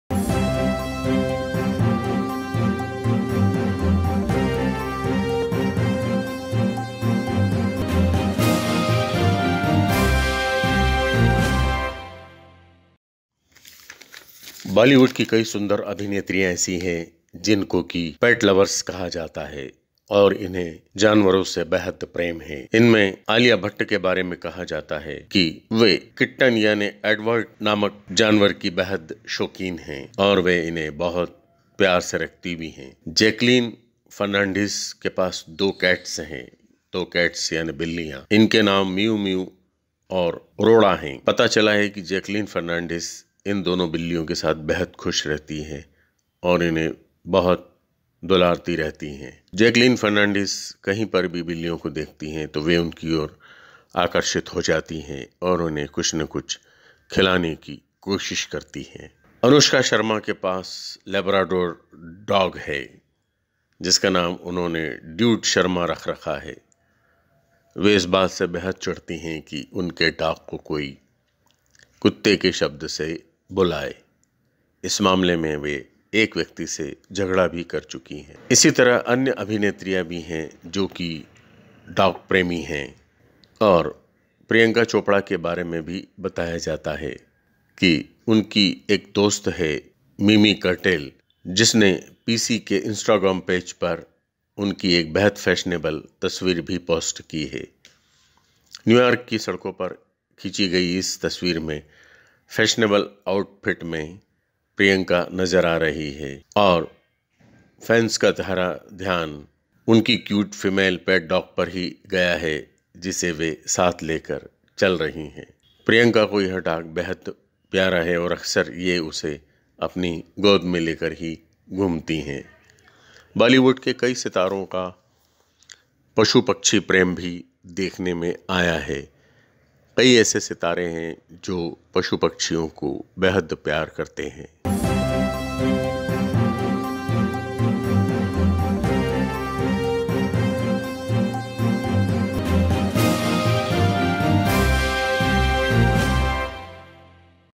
जानिए सुंदर अभिनेत्रियों को क्यों पसंद आती है बिल्लियां | Learn why beautiful actress likes cats
बॉलीवुड अभिनेत्रियों की पसंद है कई तरह के जानवर | Bollywood actresses have many types of animals